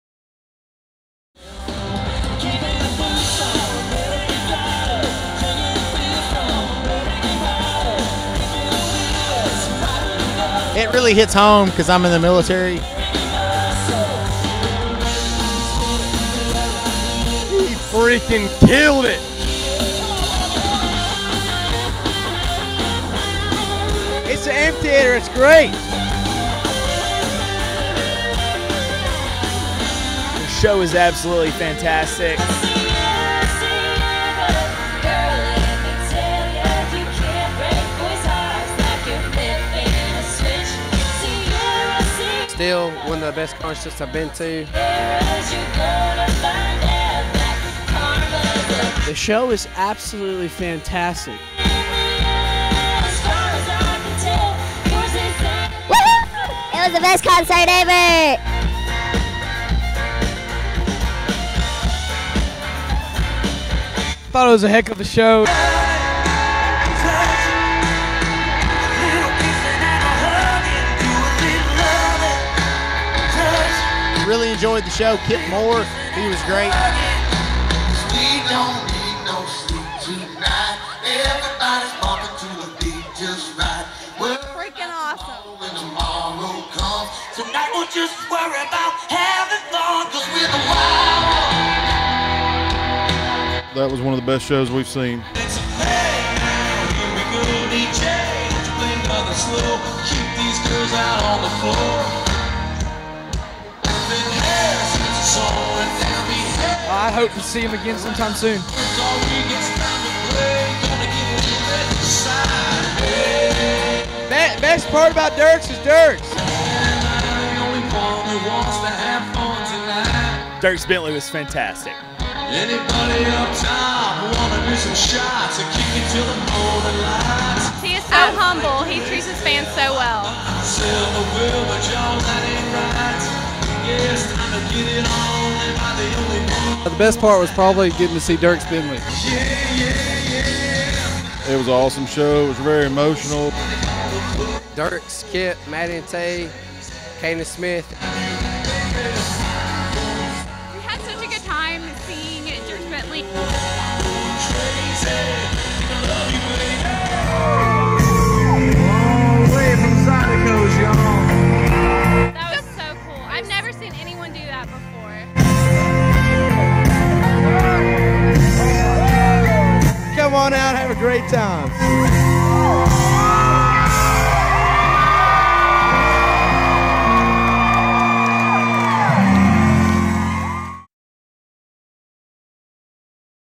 Dierks Bentley at Oak Mountain Amphitheatre 07 18 2015
4 times the fun when Canaan Smith,Maddie & Tae, Kip Moore and Dierks Bentley came to Oak Mountain Amphitheatre! Fans definitely had an awesome night.